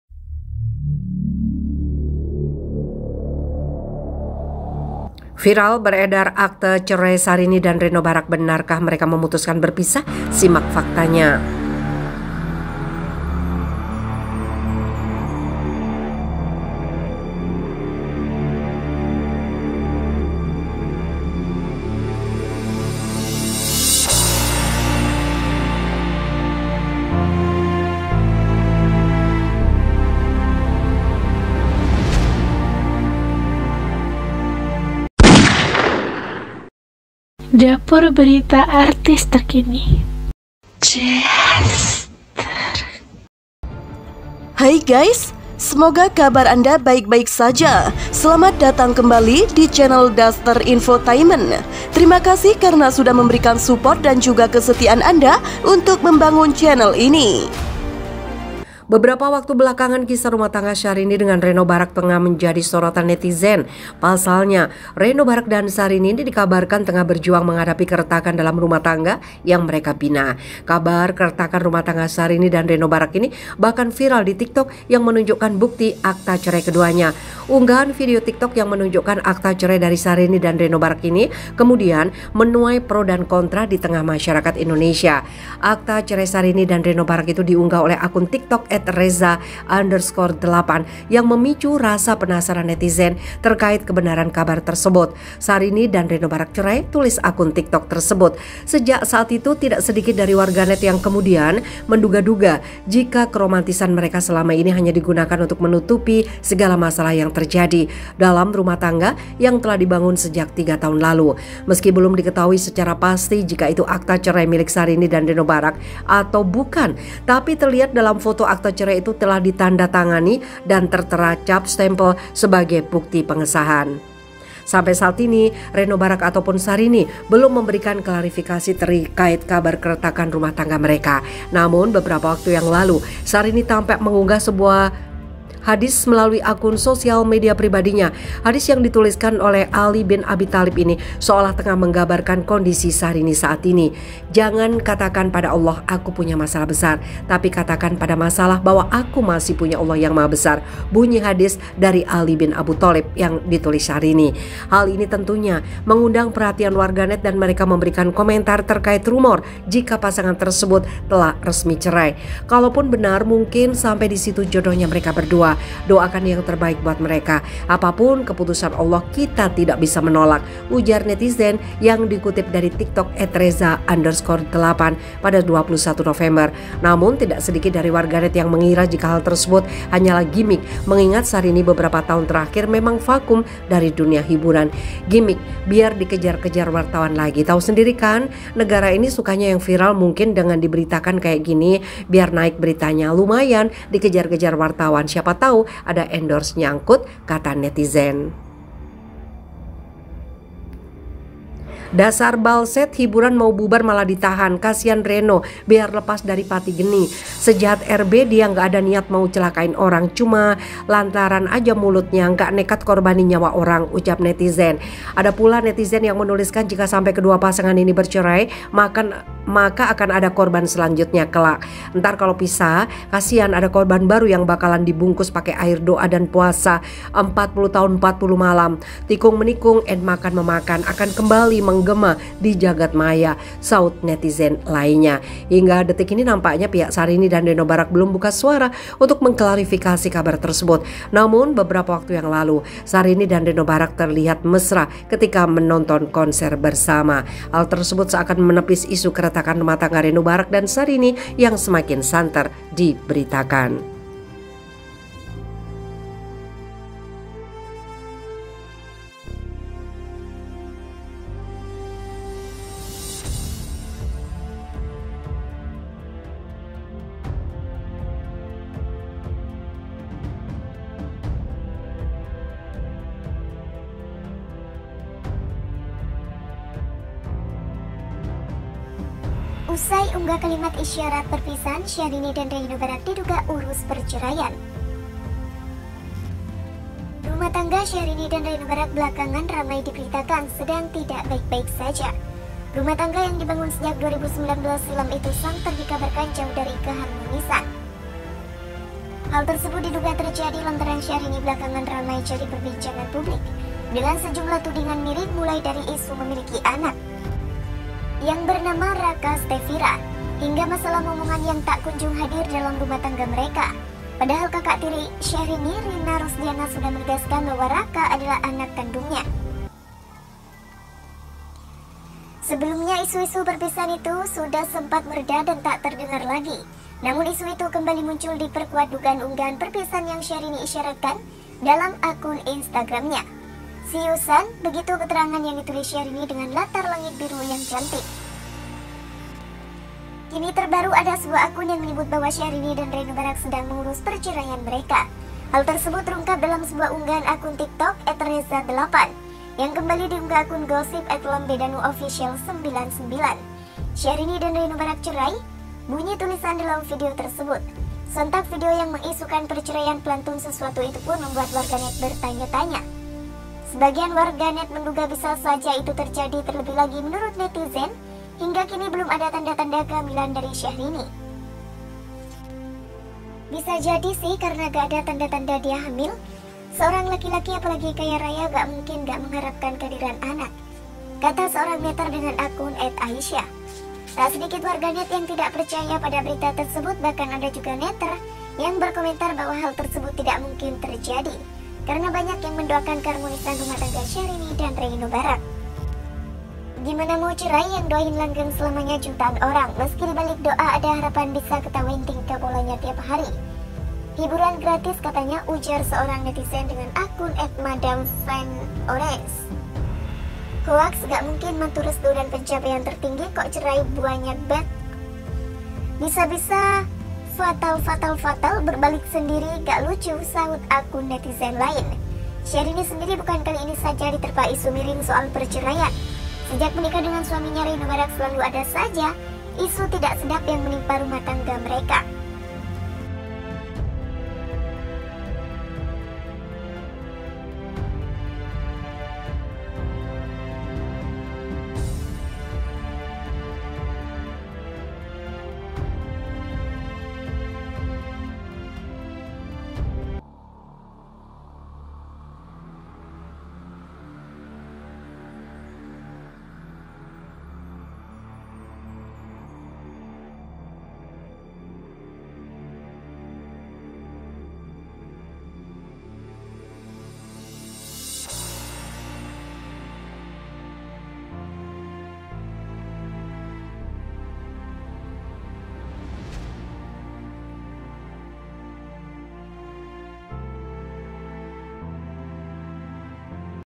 AYAH REINO MEMANG TAK SETUJUI HUBUNGAN DG SYAHRINI? DETIK2 REINO MAR4H TAK TERTAHANKAN LAGI?
gosip terbaru,
berita artis,
gosip artis,
berita artis terbaru,
berita artis hari ini,
syahrini,
syahrini dan reinobarack,
reinobarack,
lunamaya,
indonesia,